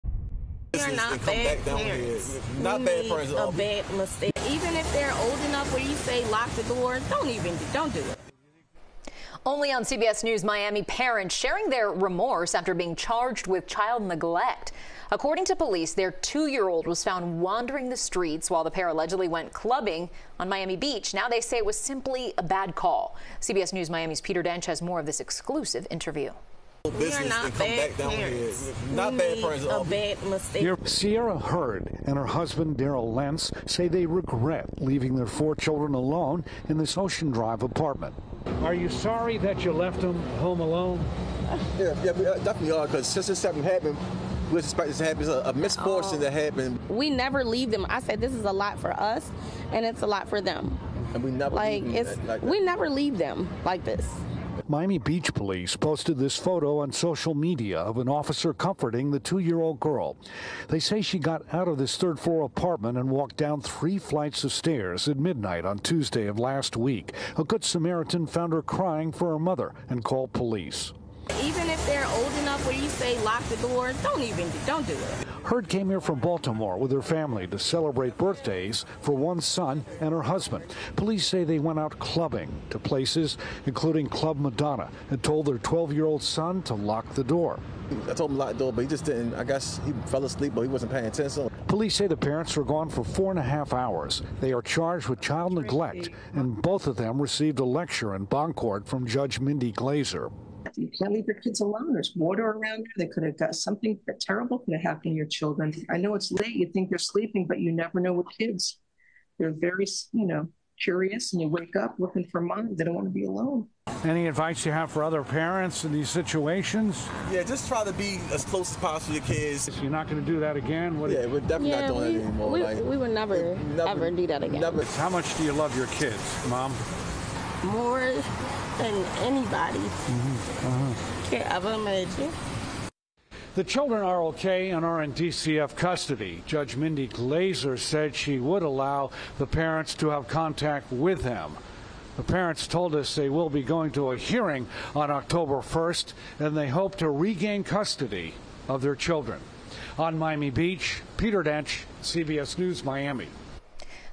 Child found wandering streets while parents went clubbing in Miami Beach
A mother and father are speaking out exclusively to CBS News Miami after police said they left their four children home alone, while they went to nightclubs on Miami Beach.  "We are not bad parents,"